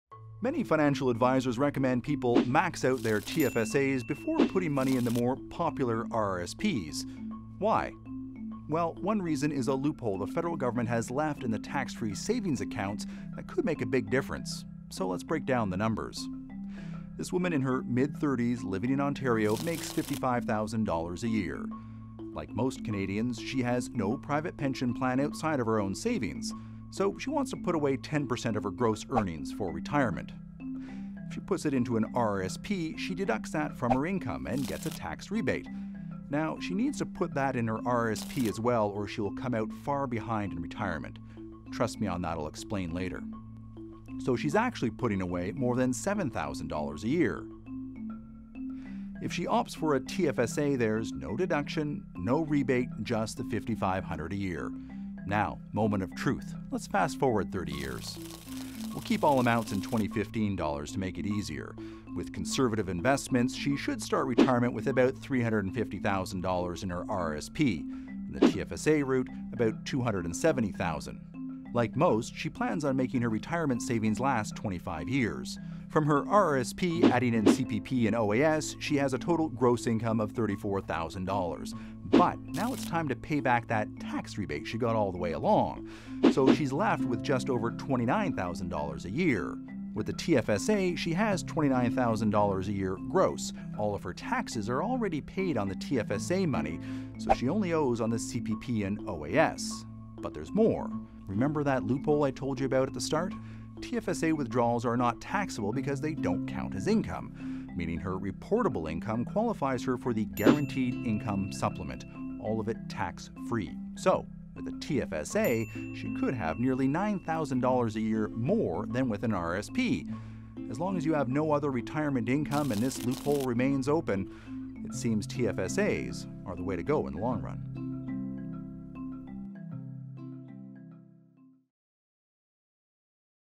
TFSAs vs. RRSPs
How are tax-free savings accounts different from registered retirement savings plans? James Fitz-Morris explains your options.